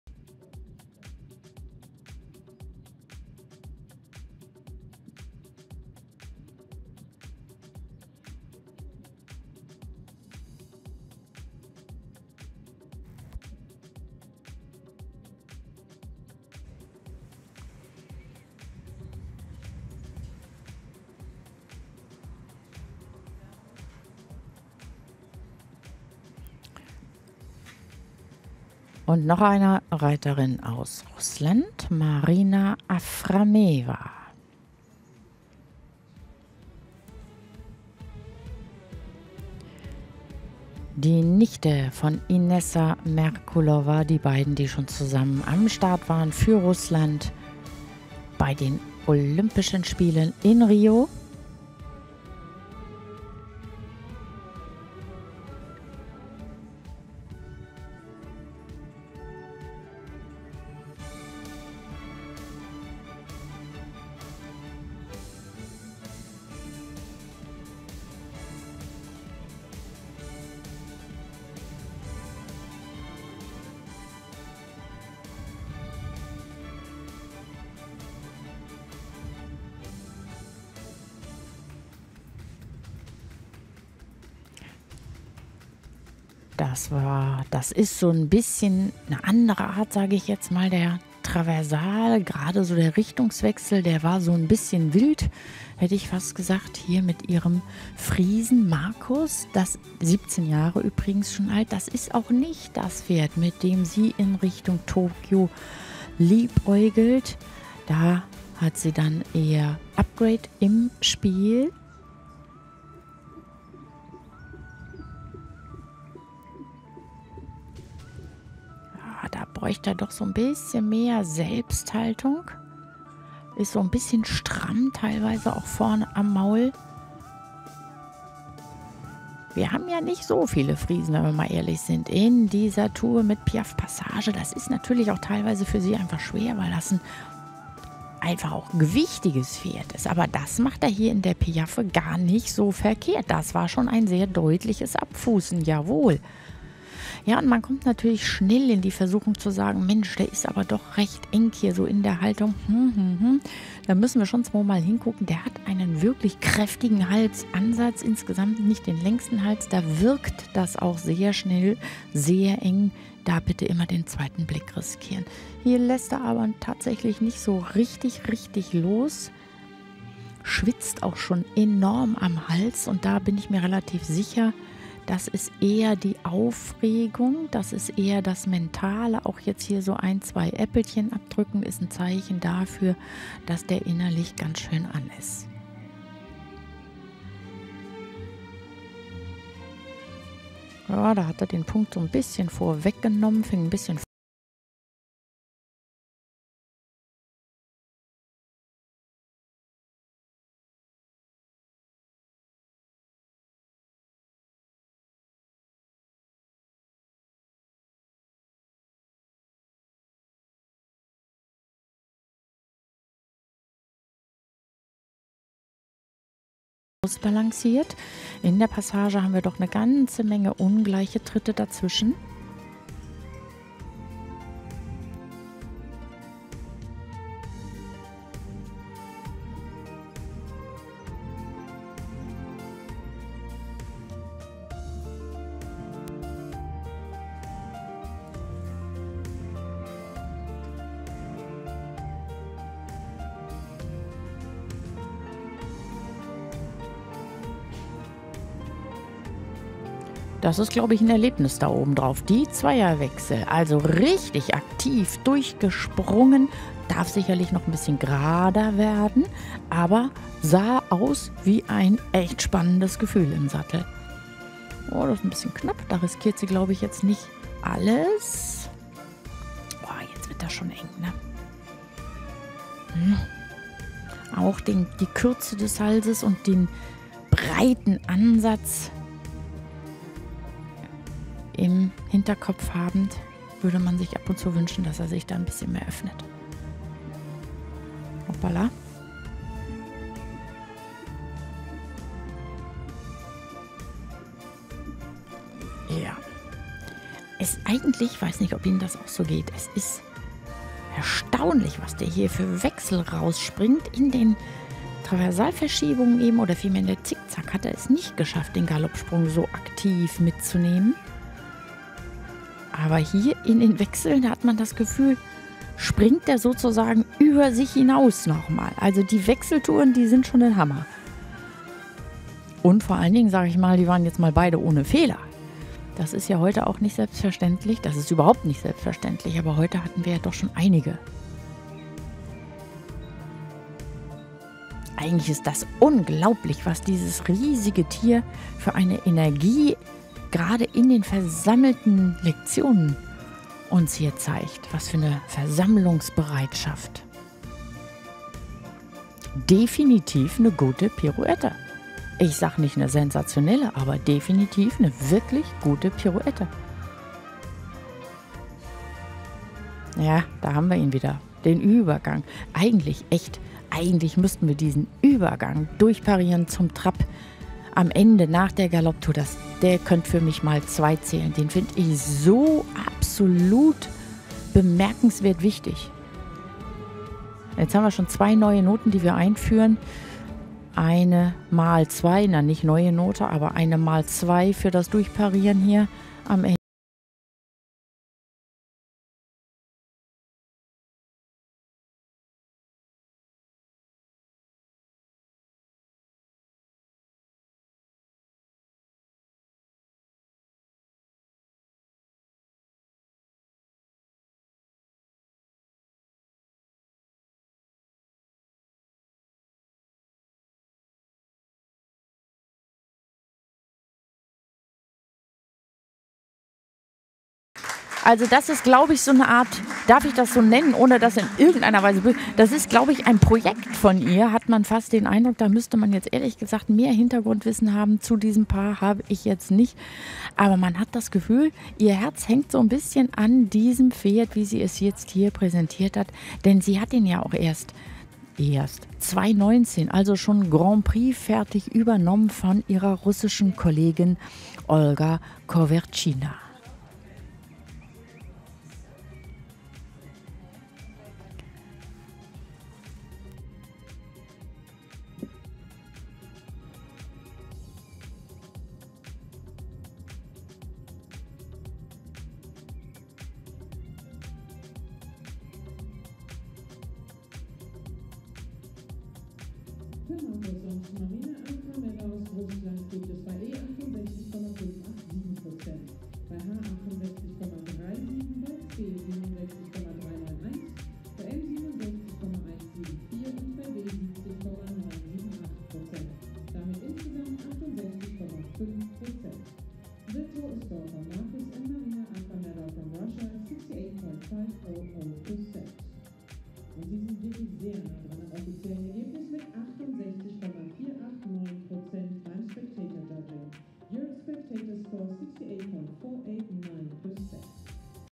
2021.06.25 Marcus & Marina, GP in Kronberg 68.5%
CDI 4* in Kronberg.
Friesian stallion Marcus in 't Rietveld (s. Beart 411) and Marina Aframeeva performed Grand Prix.
26 June 2021.